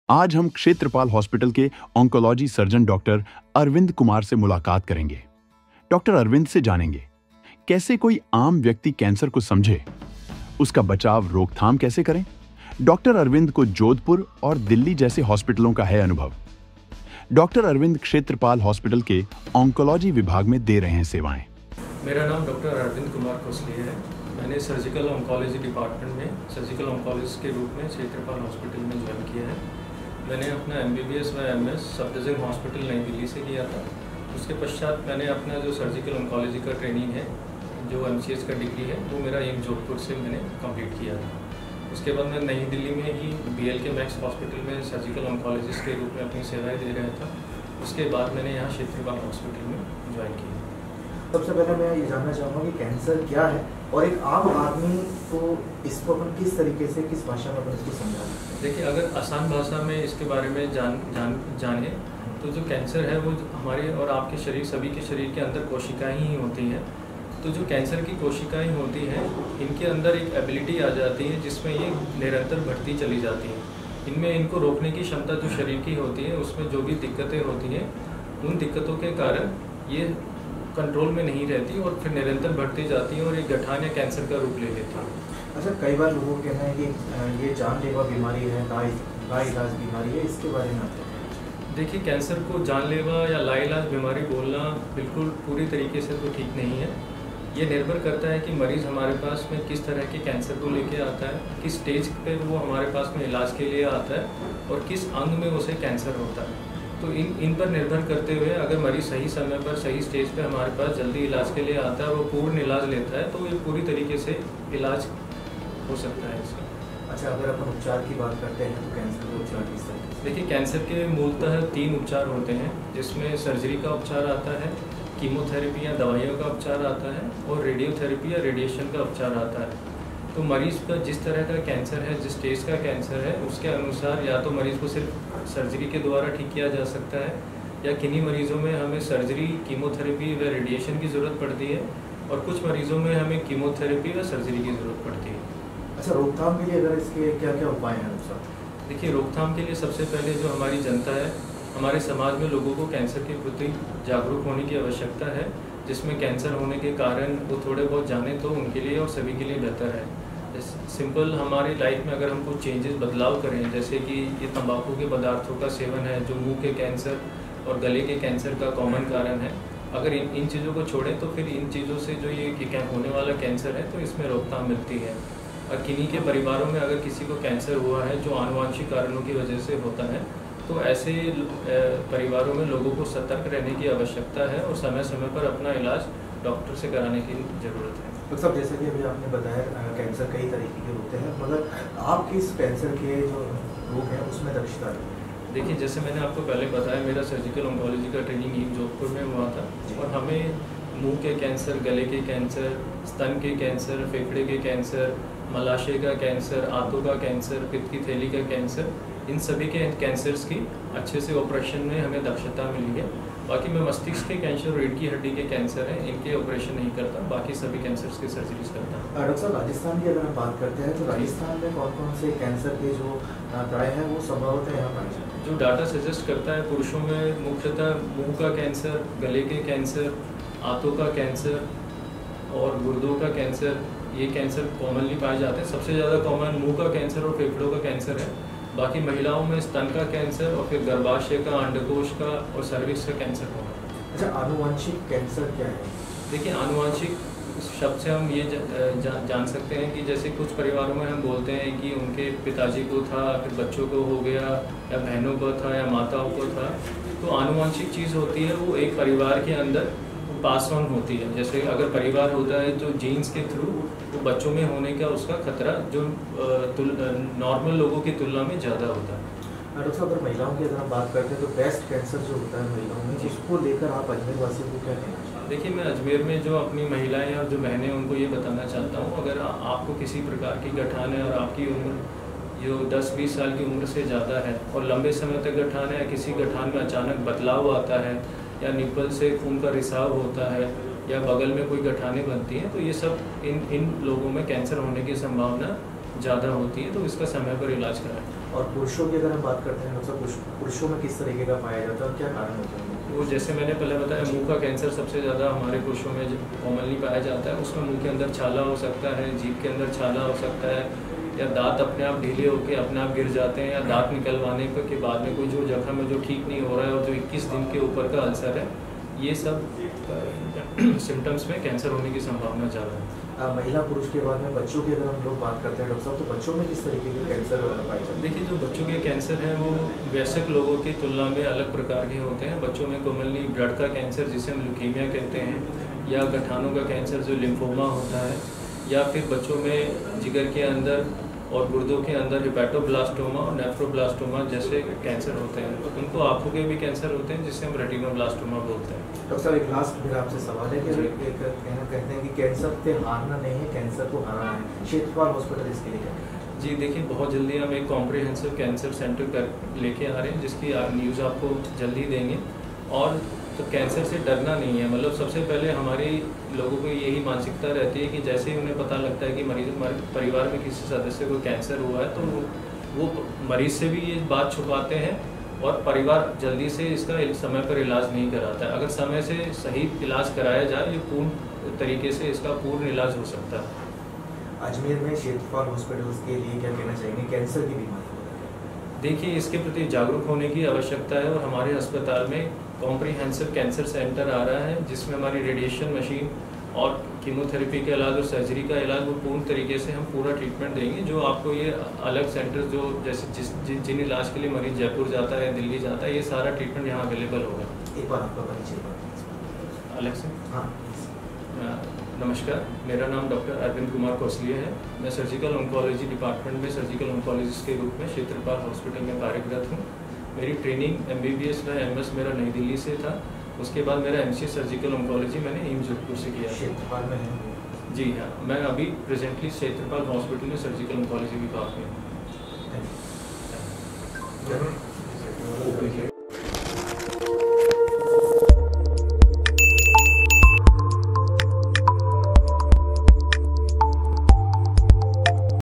Ajmer News क्षेत्रपाल हॉस्पिटल के साथ जानिए कैसे कैंसर को हराना है | MTTV
क्षेत्रपाल हॉस्पिटल के साथ जानिए कैसे कैंसर को हराना है क्षेत्रपाल हॉस्पिटल अजमेर के ऑंकोलॉजी सर्जन डॉक्टर अरविंद से आम आदमी कैंसर को समझे ,कैंसर जानलेवा बीमारी है तो उसका उपचार और बचाव कैसे? महिलाओं में ब्रेस्ट कैंसर का कोई महत्वपूर्ण कारण जानिए ,क्षेत्रपाल हॉस्पिटल के साथ जानिए कैसे कैंसर को हराना है